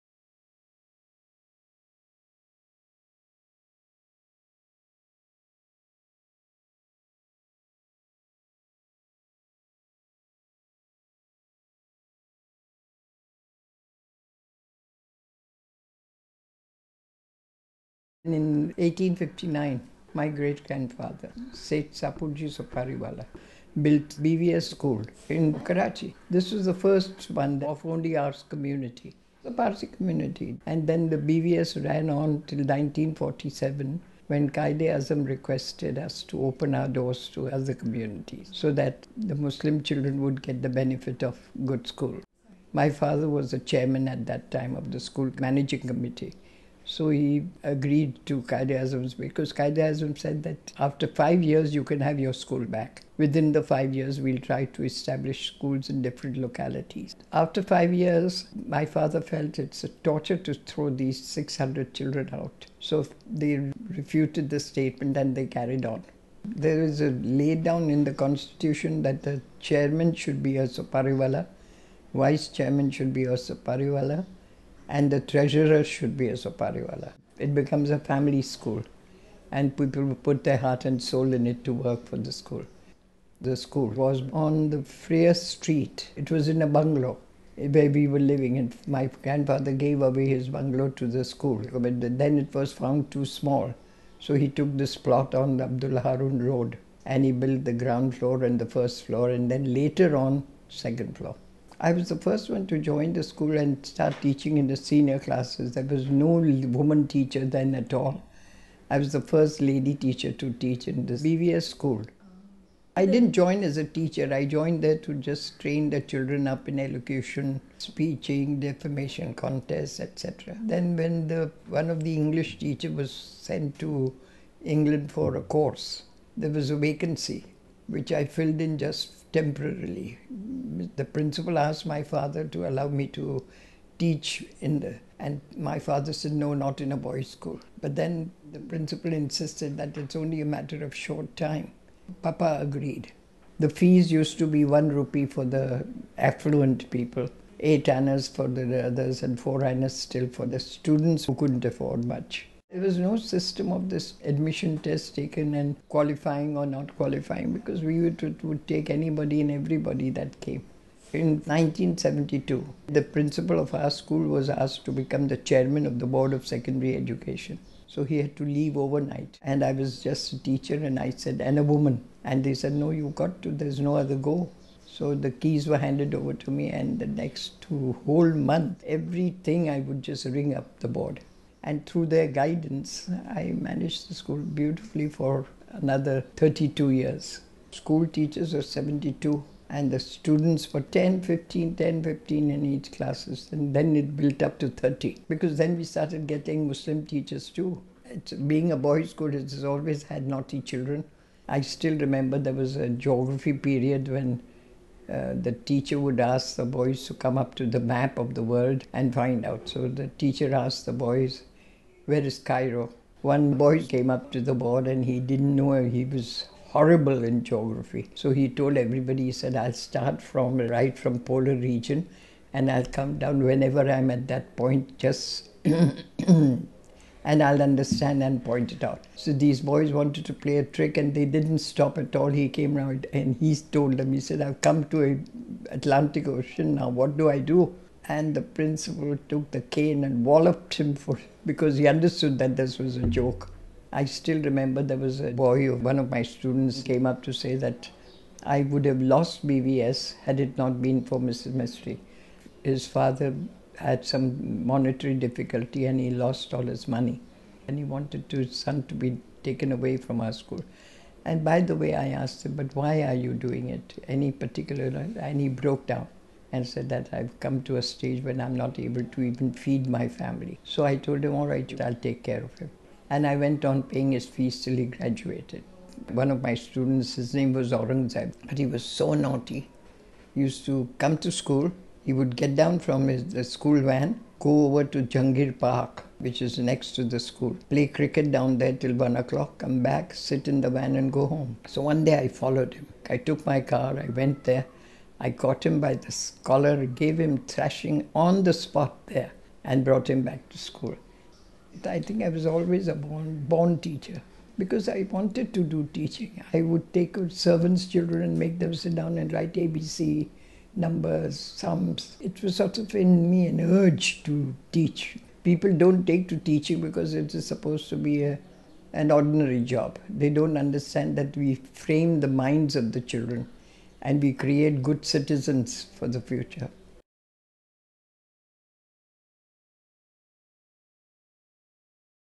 OHP - Deena Mistri Oral History
The Karachi Schools Series is a part of CAP's Oral History Project. It is a series on some of the oldest educational institutions in Karachi and the teachers and students who contributed to these and other institutions.

We recently interviewed Deena Mistri about her experience as a teacher and Headmistress at Bai Virbaiji Soparivala Parsi High School, Karachi.

Producer - Junaid Hussain -  CAP employee